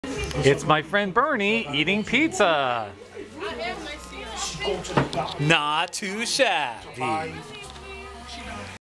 Bernie Eating Pizza - Not Too Shabby!
Pizza. Mmmm.
Luigi's Pizza on 8th Ave (near W. 25th or 26th St.)